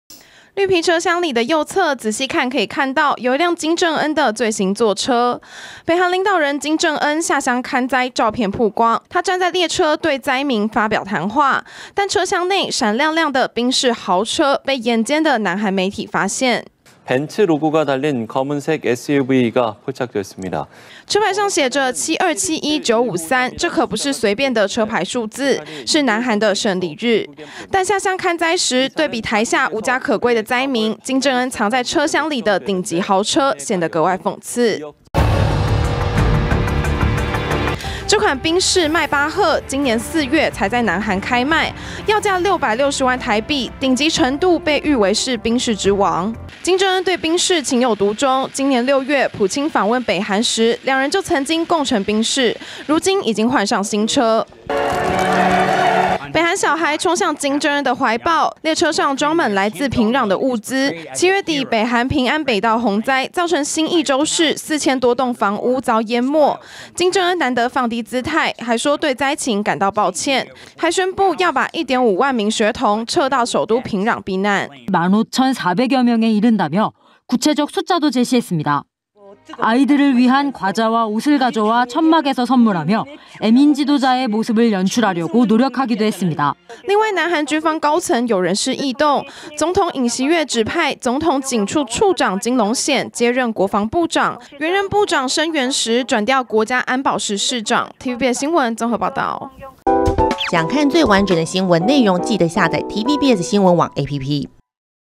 下鄉巡災藏在專列上! 金正恩「邁巴赫」新豪車露餡｜TVBS新聞
北韓發生洪患，領導人金正恩視察災區，站在專列上發表演說，南韓媒體發現，車廂內藏有一輛賓士旗下最高級品牌【邁巴赫】轎車，研判應該是金正恩的新車。這輛車要價660萬台幣，對比台下的災民，格外諷刺。